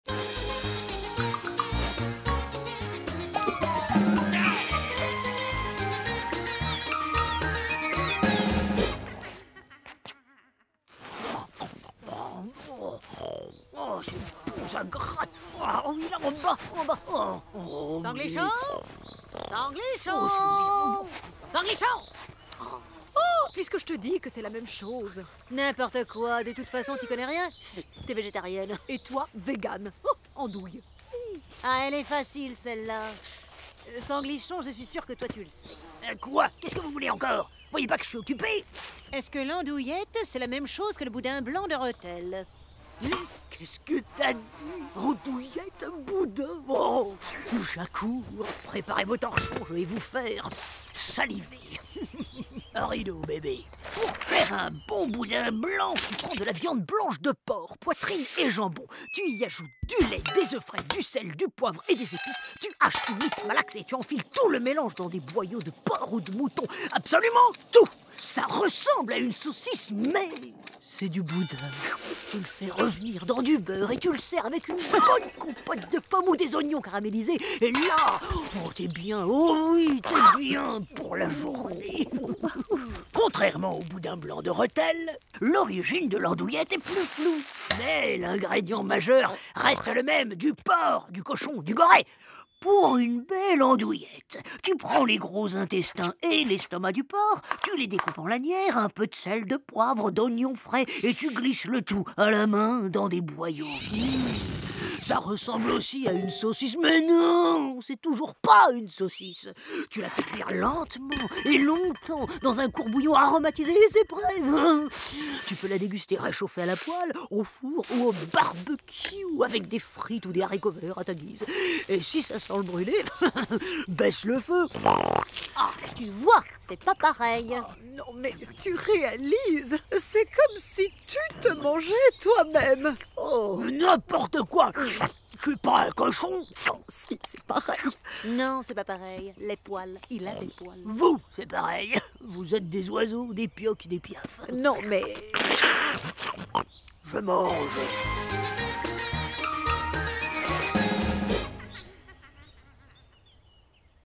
Andouillette vs boudin blanc, dans les petites histoires du Grand Est (2/30)
Connaissez-vous la différence entre l'andouillette et le boudin blanc ? Sanglichon, le sanglier de Champagne-Ardenne, va se faire un plaisir de vous l'expliquer.